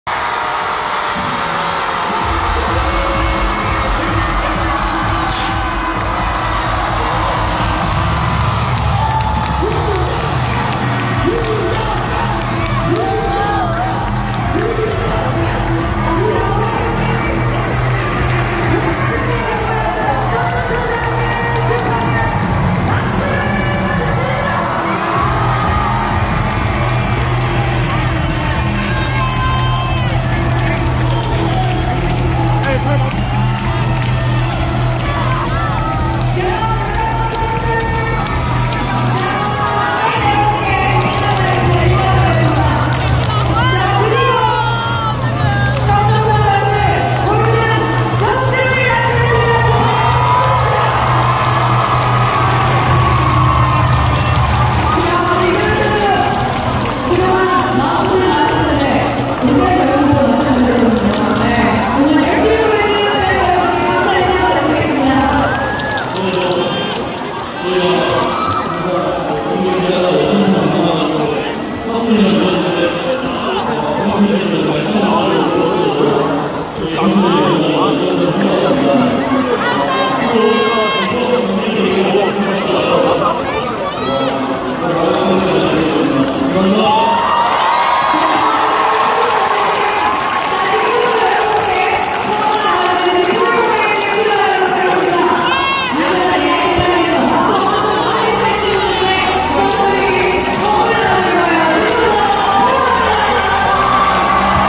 서울 삼성 썬더스 BELIEVE & ACHIEVE 승리 직관
촬영장소 : 잠실실내체육관

촬영날짜 : 2024년 11월 2일 오후 5시 49분